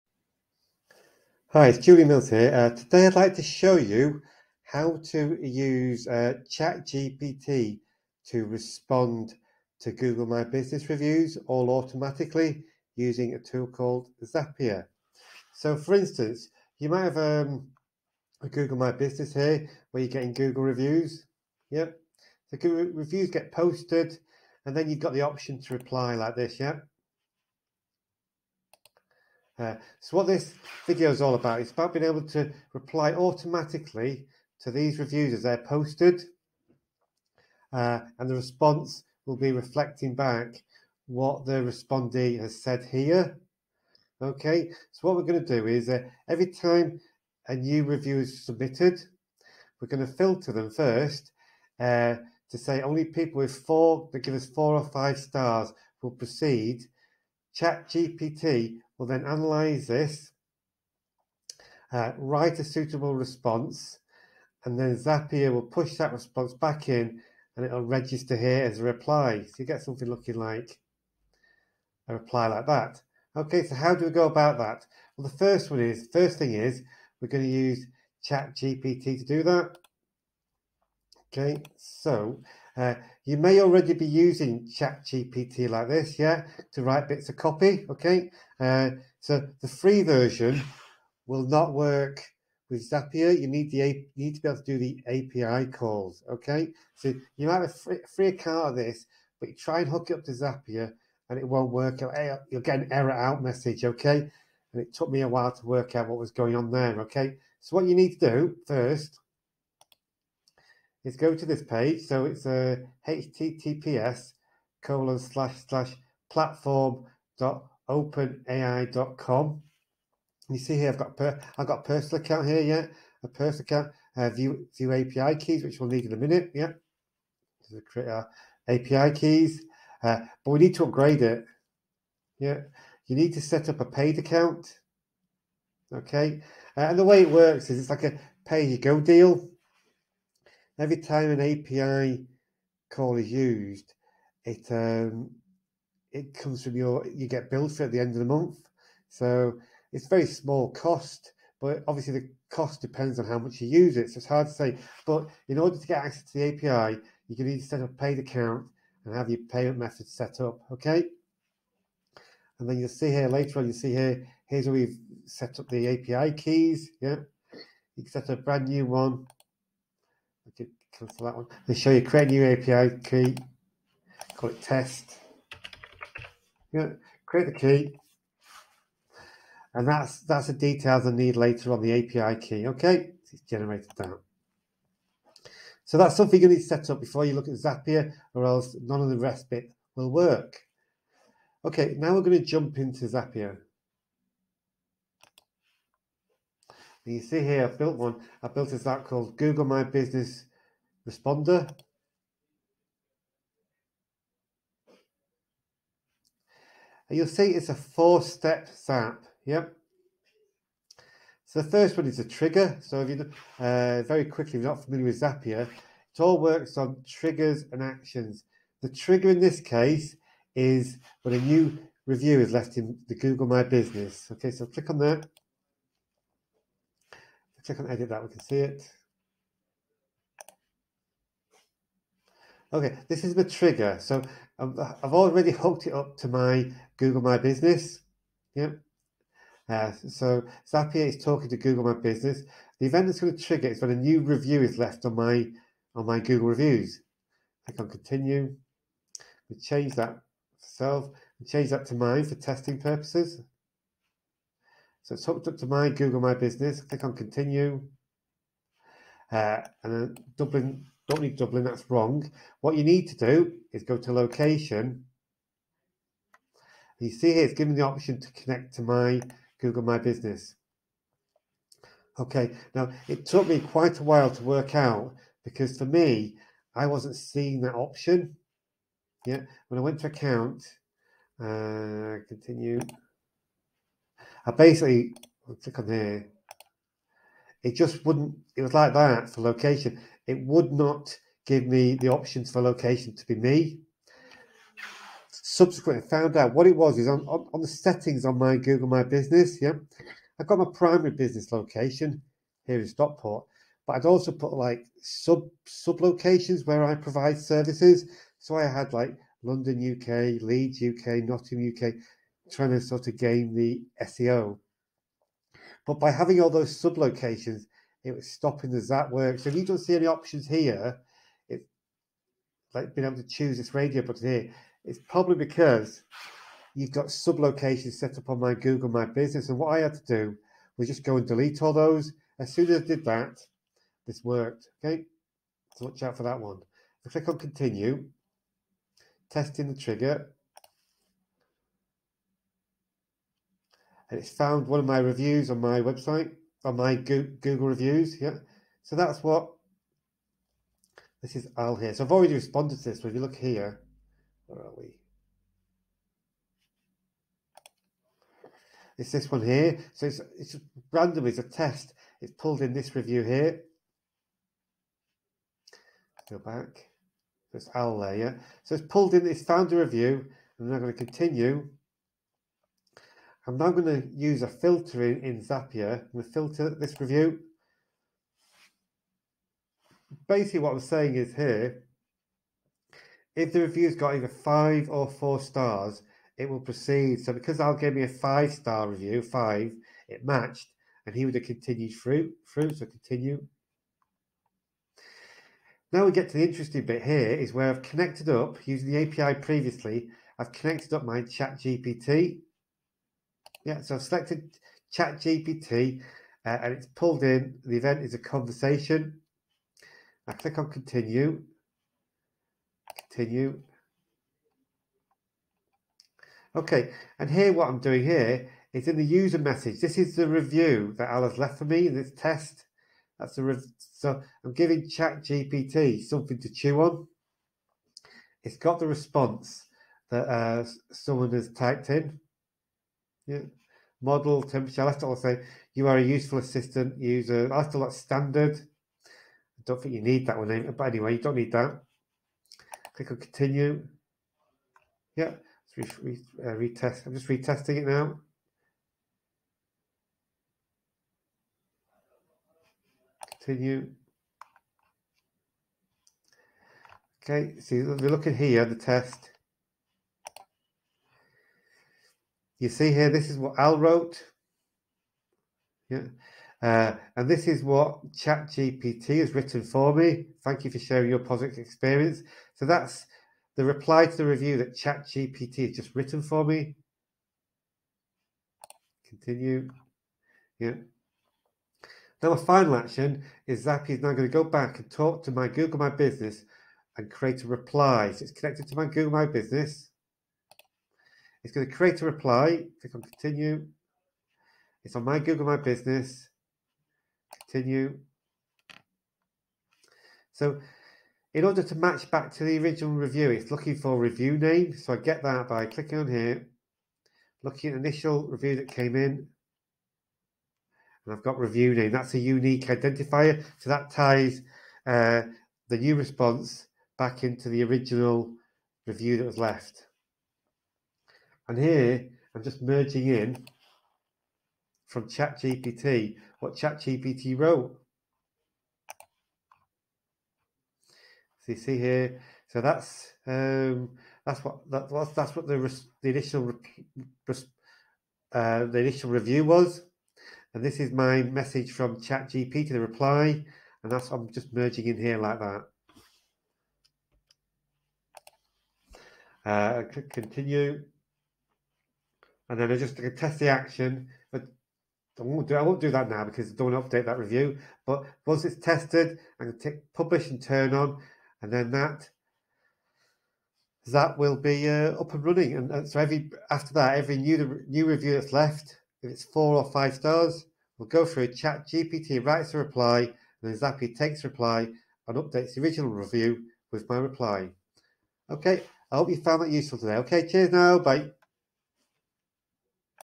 How to automatically reply to Google Reviews using ChatGPT AI and Zapier
If you are generating Google reviews on Google My Business then it can be a smart move to reply to Google My Business reviews. But it is a manual chore. So this is an ideal use case for ChatGPT.

In the video below these are the stages we go through.

Someone leaves a review on Google My Business.
If they leave a 4 or 5-star review then we filter them with Zapier.
Zapier then pushes the details of the review into ChatGPT
ChatGPT writes a unique reply to the review.
ChatGPT pushes the reply message back into Zapier.
Zapier updates the Google review with the reply generated by Chat GPT.

So in the video below, I go through the Zapier setup to do this. I assume you are familiar with using Zapier.

So this solution in the above video accommodates 4 or 5-star reviews. As smart as ChatGPT is I would not be comfortable with having it reply automatically to any 1,2 or 3-star reviews. In these instances, I feel it is better for a human to respond who can act/reply appropriately. This too can be accommodated using Zapier. In this instance we would create a second Zap in Zapier - and it would filter so we only deal with 1,2 or 3-star reviews. When we get one of these reviews I would have Zapier send an email to my client to tell them to look at this particular Google review and manually intervene and respond to what has been said.

If you have any questions I would be happy to jump on a quick call to discuss and give you some pointers.